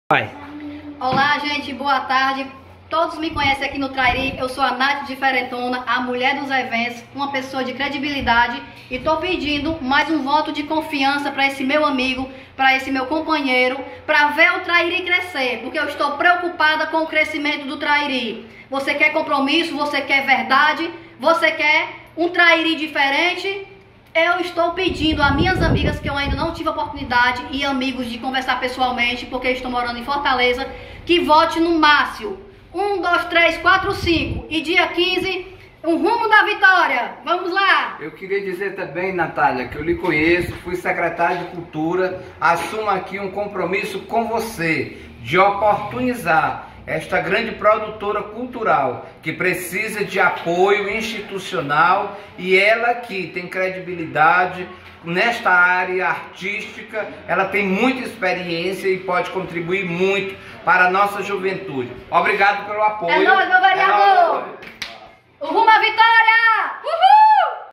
Natália Lucas Declara apoio ao Vereador Márcio Alves
A produtora cultural Natália Lucas Declara apoio ao Vereador Márcio Alves da Munguba e pede voto pro amigo.